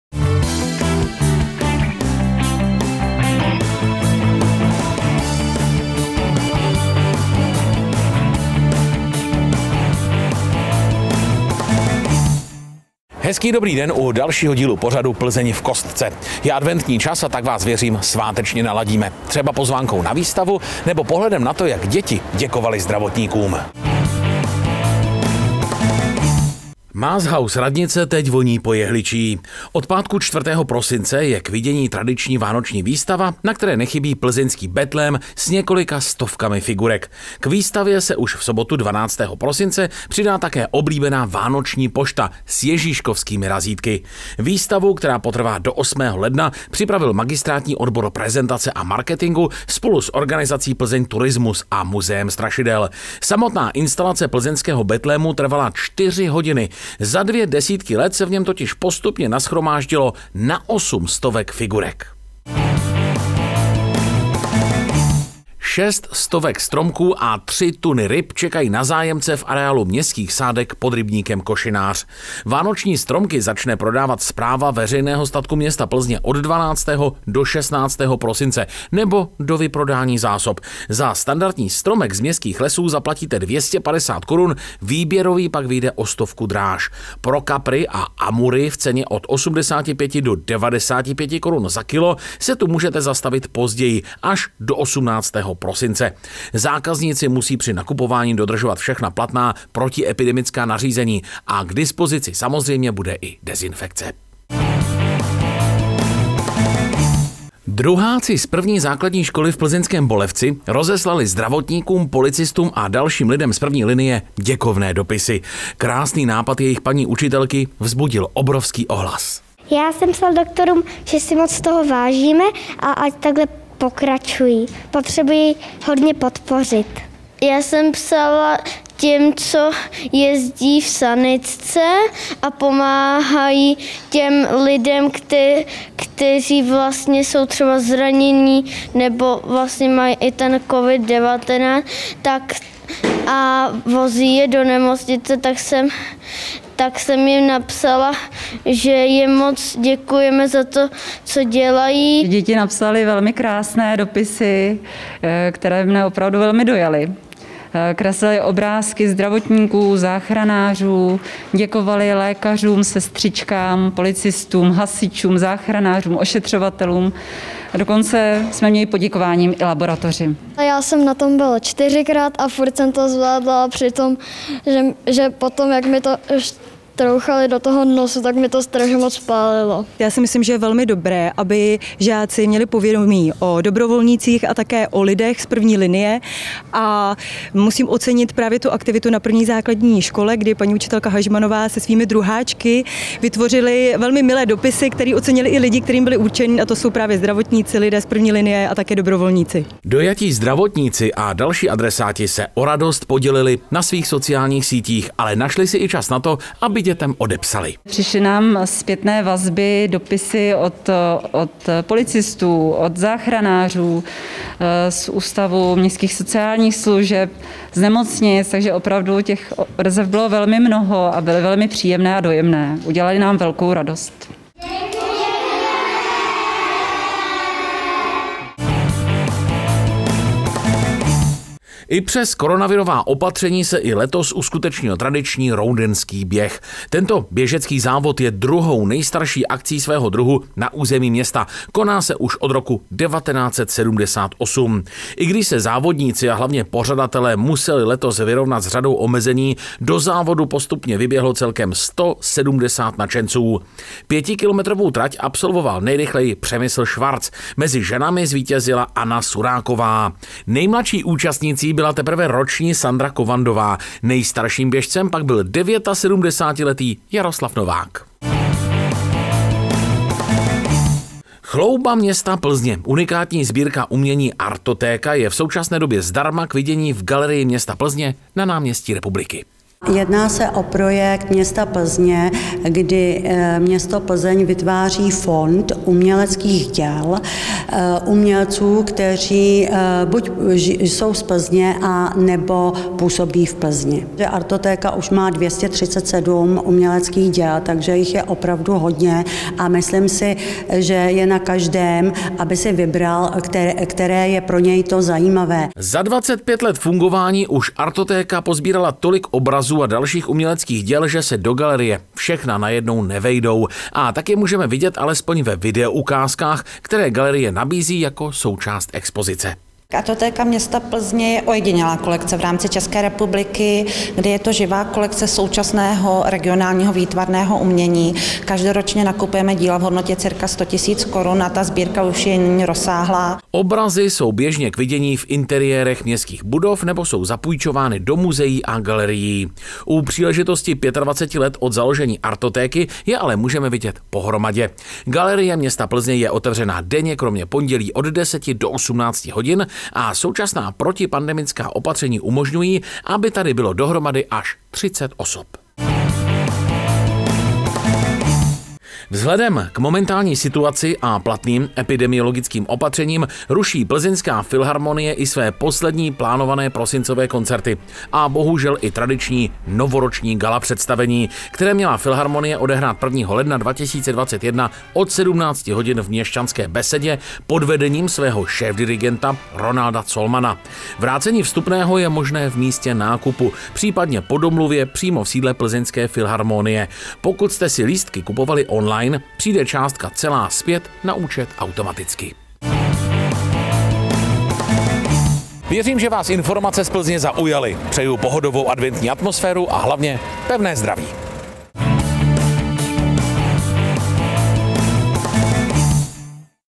Plzeň v kostce
Reportáže:
Mázhauz radnice zdobí betlém
Město zahájí prodej stromků a ryb
Děti poděkovaly obrázky nejen zdravotníkům
Roudenský běh přilákal téměř dvě stovky účastníků
Plzeň představuje svou artotéku
Filharmonie vrací lístky, zrušila i novoroční koncert